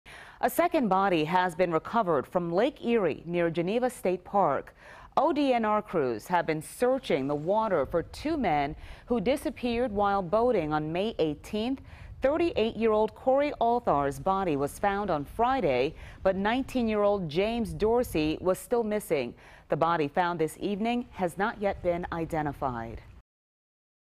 Second boater`s body recovered from Lake Erie near Geneva State Park, ODNR says
A second body was recovered from Lake Erie near Geneva State Park on Sunday night.